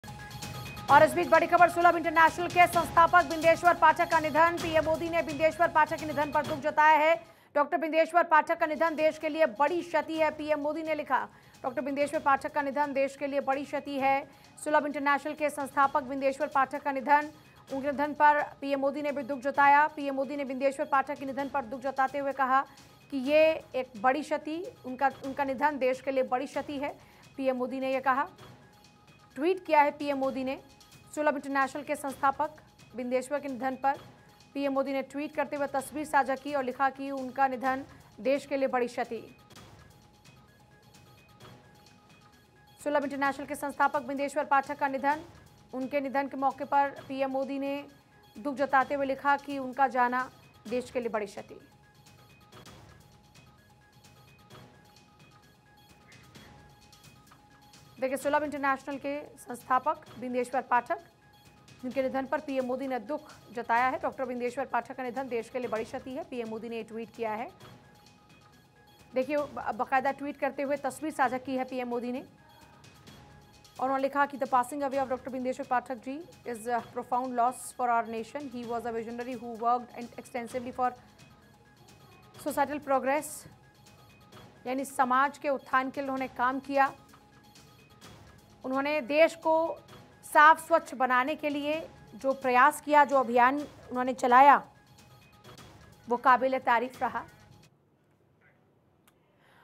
सुलभ इंटरनेशनल के संस्थापक Bindeshwar Pathak का निधन, PM Modi ने बिंदेश्वर पाठक के निधन पर दुख जताया
सुलभ इंटरनेशनल के संस्थापक बिंदेश्वर पाठक का निधन
पीएम मोदी ने बिंदेश्वर पाठक के निधन पर दुख जताया
'डॉ. बिंदेश्वर पाठक का निधन देश के लिए बड़ी क्षति'- PM मोदी